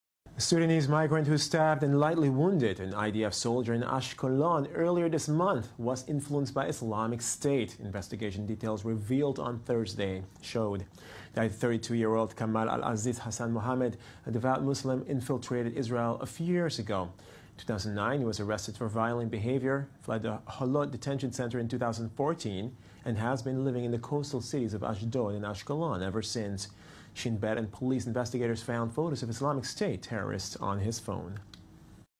Sudanese migrant who stabbed Israeli IS-related
A Sudanese migrant who stabbed and wounded an IDF soldier in Ashkelon earlier this month was influenced by IslamicState , investigation details revealed on Thursday showed. The 32-year-old Kamal al-Aziz Hassan Mohammed, a devout Muslim, infiltrated Israel a few years ago.